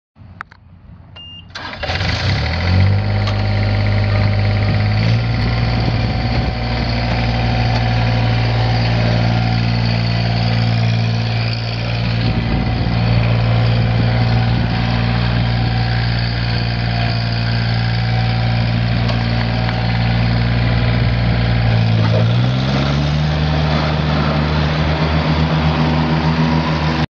John Deere 1445 lawn mower for sale at auction | bidding closes September 4, 2019
John Deere 1445 lawn mower
2,273 hours on meter
72" cut
Yanmar 3TNV82A-D three cylinder diesel engine
Hydrostatic transmission
OROPS
John Deere 75"W mower deck
Serial DM7203X211303
Hydraulic front deck lift
23x10.50-12 front tires
18x8.50-8 rear tires
Serial TC1445D050073
It's located in Iola, KS, and we're selling it to the highest bidder regardless of price.